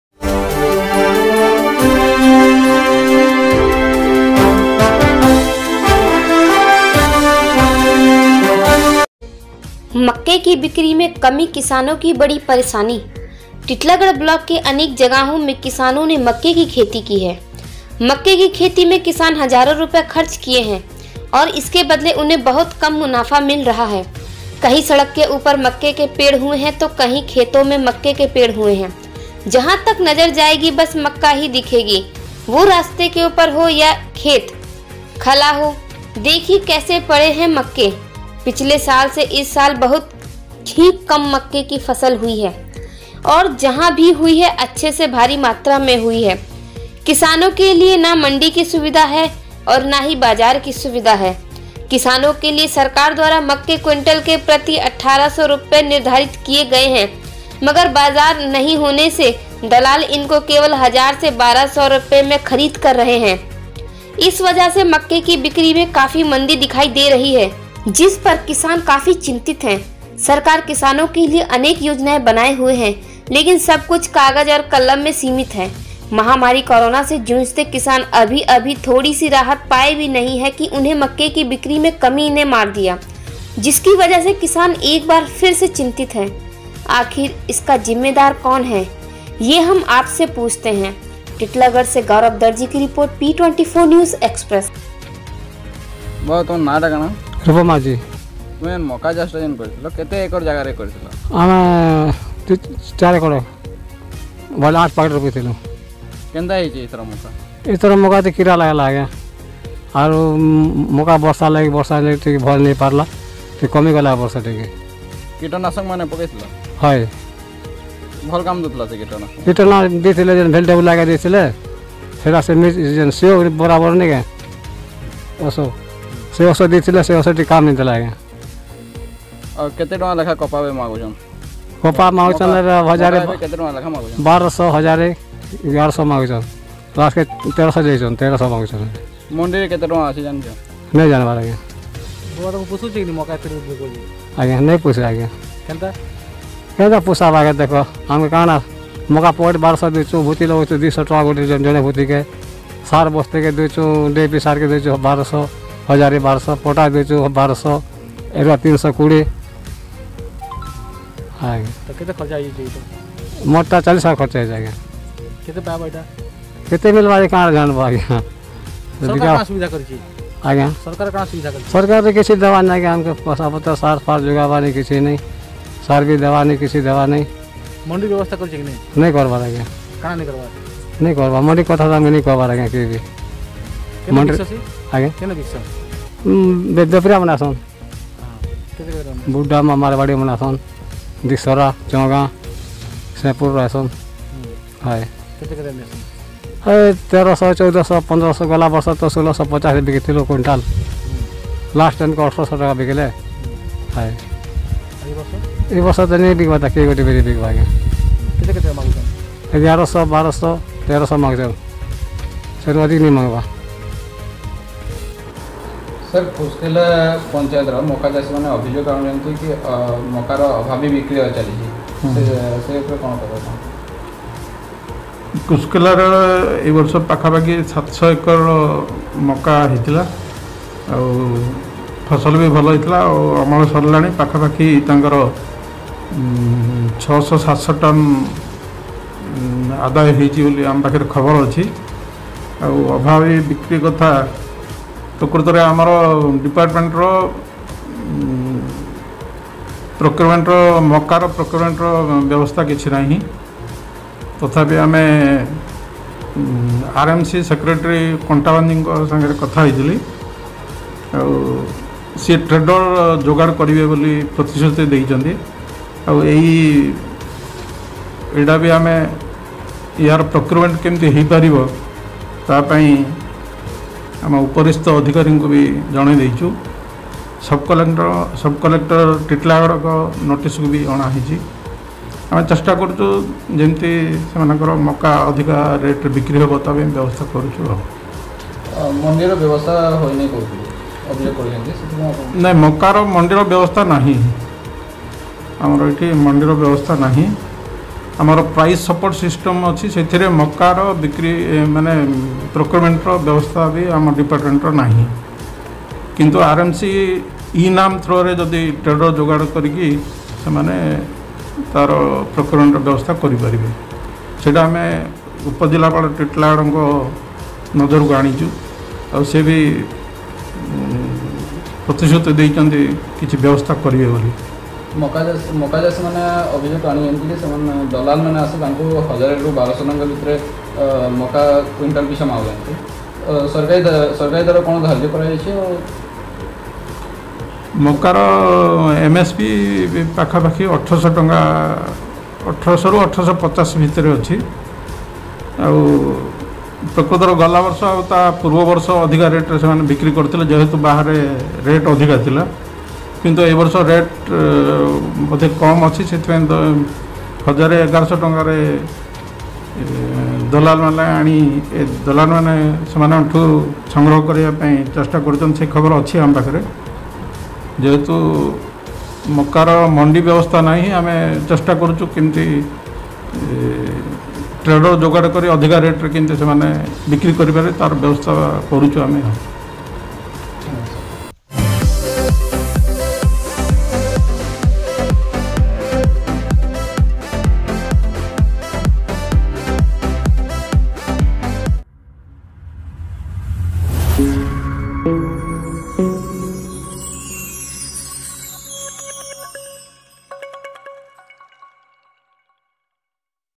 मक्के की बिक्री में कमी किसानों की बढ़ी परेशानीP24News Express
टिटिलागढ़ ब्लॉक के अनेक जगहों में किसानों ने मक्के की खेती की है। मक्के की खेती में किसान हजारों रुपए खर्च किए है और इसके बदले उन्हें बहुत ही कम मुनाफा मिल रहा है।कहीं
सड़क के उपर मक्के पड़े हुए है तो कहीं खेतो में मक्के पड़े हुए है। जहां तक नजर जाएगी बस मक्का ही दिखेगी वो रास्ते के ऊपर हो या खेत , ख़ला हो। देखिए कैसे पड़े हुए है मक्के। पिछले साल से इस साल बहुत् ही कम मक्के की फसल हुई है। और जहां भी हुई है अच्छेसे भारी मात्रा में हुई हैं । किसानों के लिए ना मंडी की सुविधा है ओर ना ही बाजार की सुविधा। किसानों के लिए सरकार द्वारा मक्के क्विंटल के प्रति 1800 रुपए निर्धारित किए गए हैं । मगर बाजार नहीं होंने से दलाल इनको केवल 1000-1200 रुपए में खरीद कर रहे है। इस वजह से मक्के की बिक्री में काफी मंदी दिखाई देरहि है ।जिसपर किसान काफी चिंतित है। सरकार किसानों के लिए अनेक योजनाएं बनाएं हुए हैं लेकिन सब कुछ कागज और कलम में सीमित है। महामारी कोरोना से जूझते किसान अभी अभी थोड़ी सी राहत पाए भी नहीं की उन्हें मक्के की बिक्री में कमी ने मरदिया जिसकी वजह से किसान एकबार फिर से चिंतित है। आखिर इसका जिम्मेदार कौन है ये हम आपसे पूछते है ।P24News Express

टिटिलागढ़ से गौरव दर्जी की रिपोर्ट।